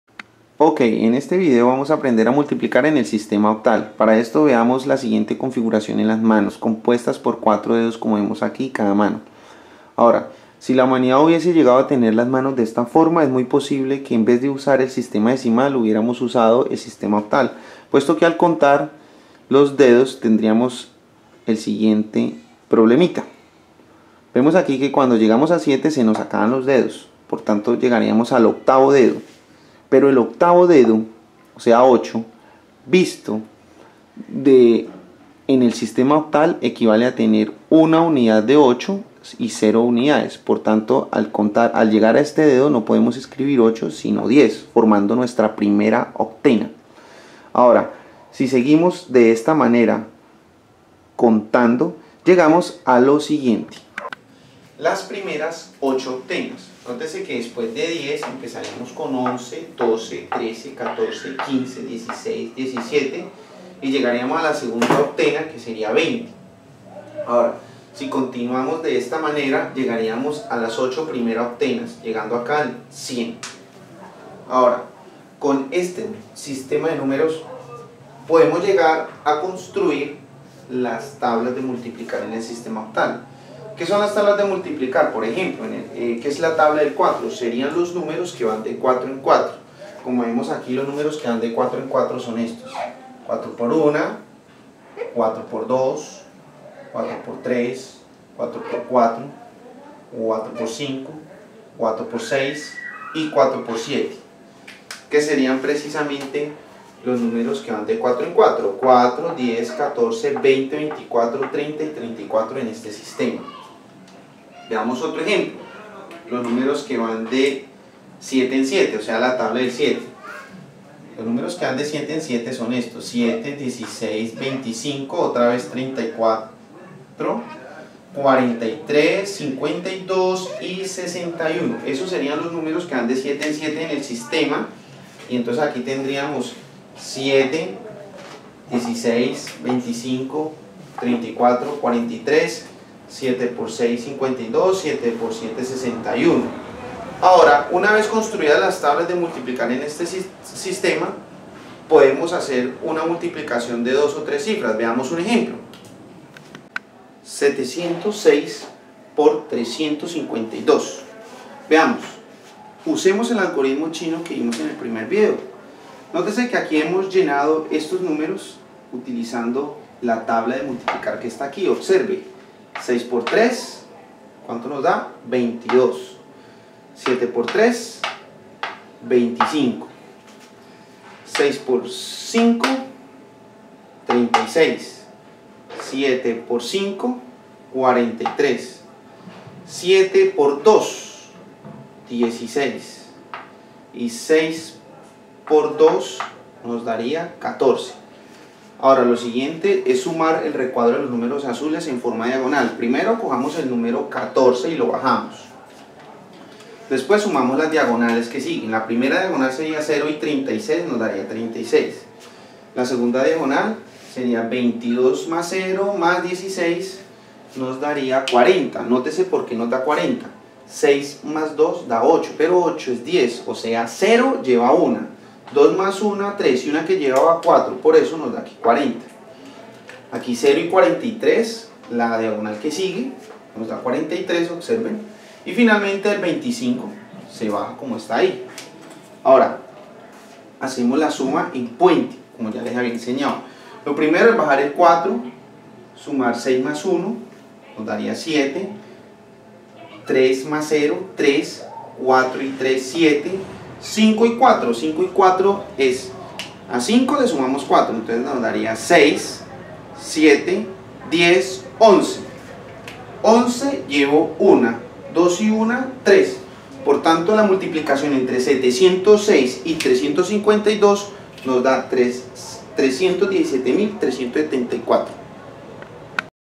Sistema Octal III - (La multiplicación)
En este vídeo vamos aprender a multiplicar en el sistema octal.